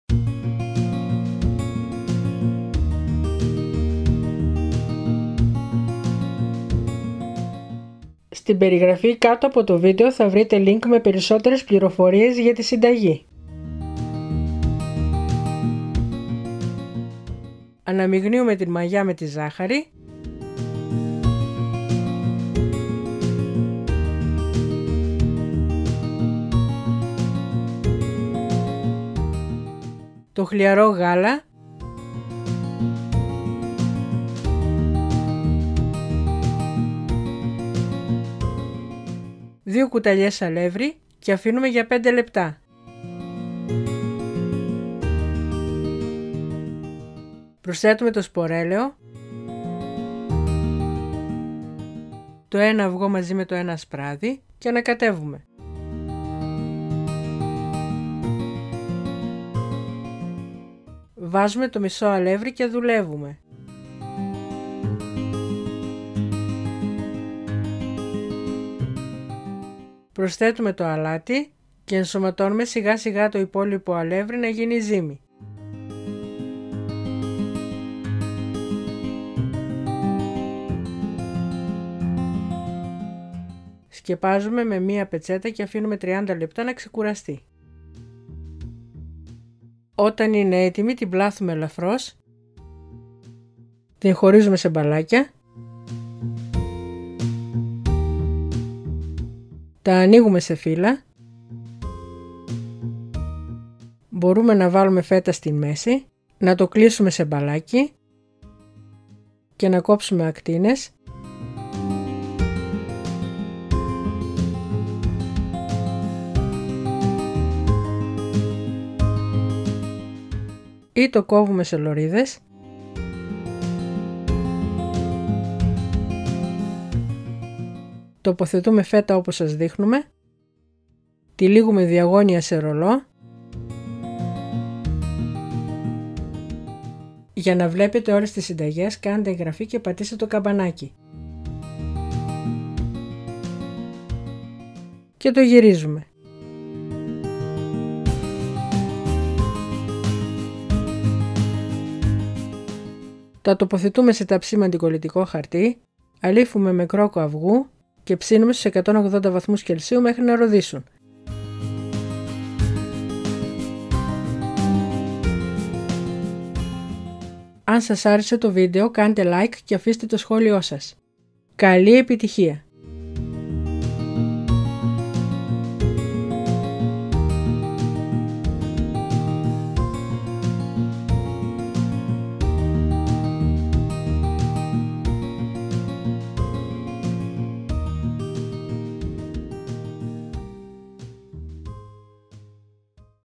Τυροπιτάκια με 2 σχέδια (Acma) | Foodaholics
Τυροπιτάκια με 2 σχέδια (Acma) | Turopitakia me 2 sxedia (Acma) | Foodaholics

 Συστατικά:

Περίπου 600 γρ αλεύρι
 200 ml γάλα
 150 ml σπορέλαιο
 8 γρ ξηρή μαγιά
 2 αυγά
 1 κουταλιά ζάχαρη
 Λίγο αλάτι
 Φέτα

  Approximately 600 g of flour
  200 ml of milk
  150 ml of seed oil
  8 g of dry yeast
  2 eggs
  1 tablespoon of sugar
  A pinch of salt
  Greek white cheese feta